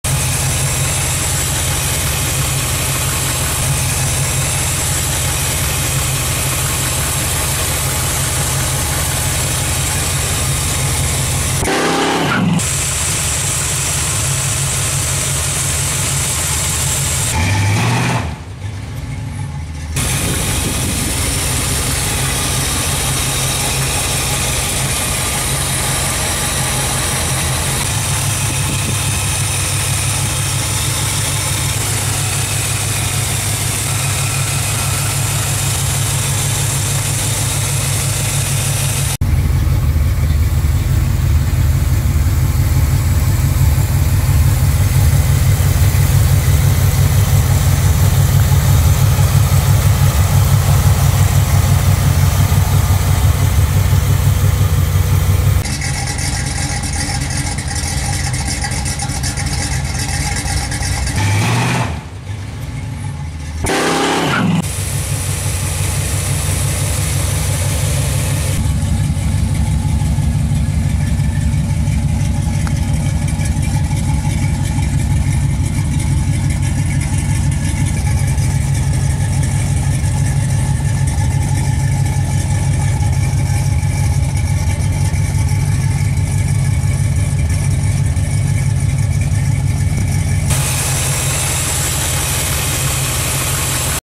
Camaro. Ultra Temp Wires. Wire Loom, Ignition Wire Organizer. Keeps Wires Organized. MSD Heli Core.
Keep your wires neat and organized. Dress up your engine and keep a clean, organized look. MSD Heli-Core Solid Suppression Ignition Wires.